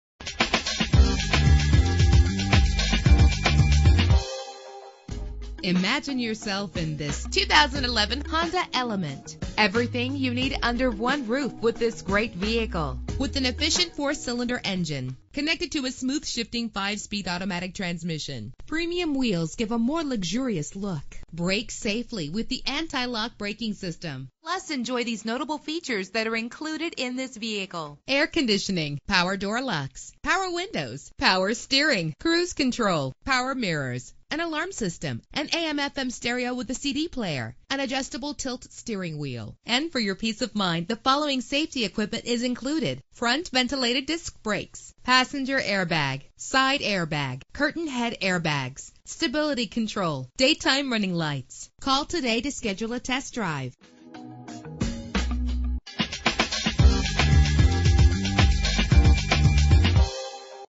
2011 Honda Element - Laurel MD
Year: 2011
Make: Honda
Model: Element
Trim: EX
Engine: 2.4 liter inline 4 cylinder 16 valve
Transmission: 5-Speed Automatic
Color: Dark Gray
Mileage: 5
Address:
3371 Laurel Fort Meade Road